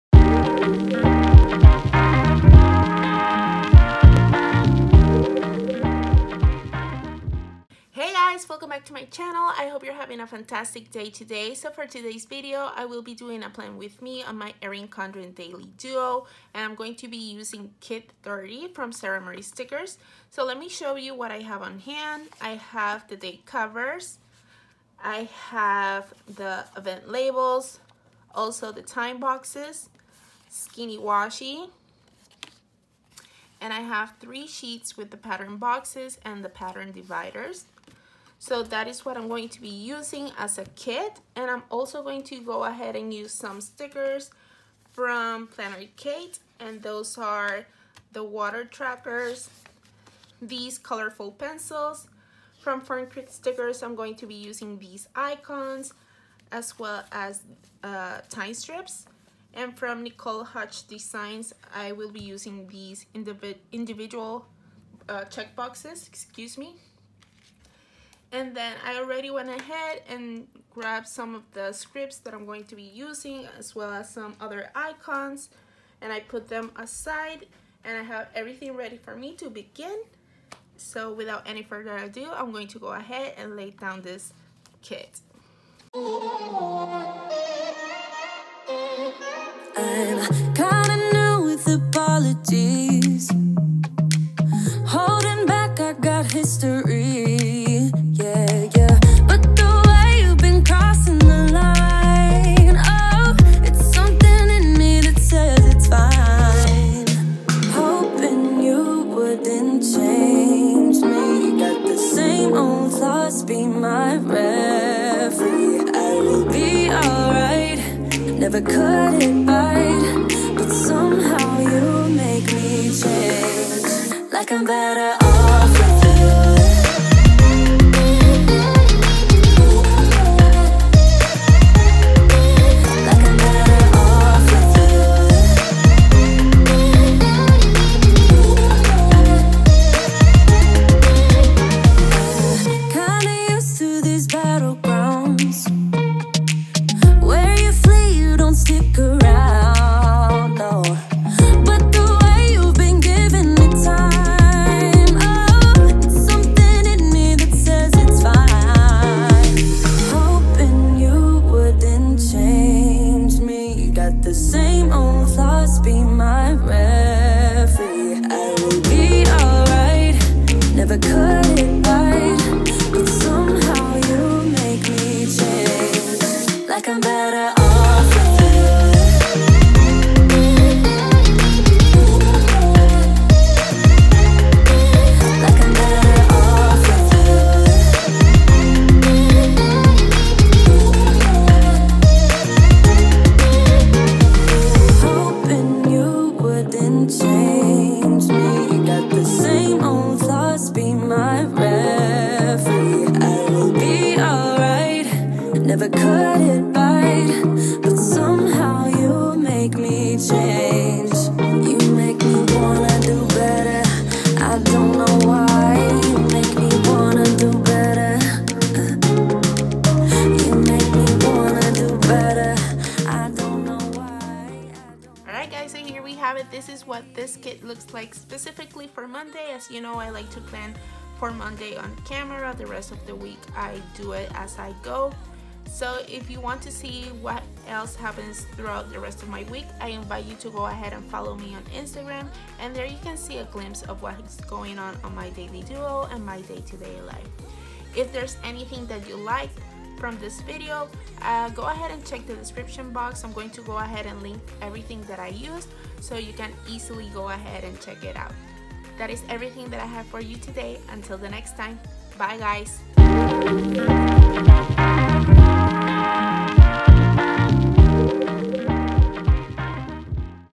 ERIN CONDREN DAILY DUO PLAN WITH ME FEB. 22ND | SARA MARIE STICKER | GABRIELA PLANS IT
In this video, I'll be sharing with you a plan with me in my Erin Condren Daily Duo planner using kit 30 from Sara Marie Stickers. I hope you enjoy the video!

SARA MARIE STICKERS KIT 30
PLANNERKATE PENCILS S-206 AND WATER TRACKER S-742
NICOLE HUTCH DESIGNS INDIVIDUAL CHECKBOX F0009
MUJI PEN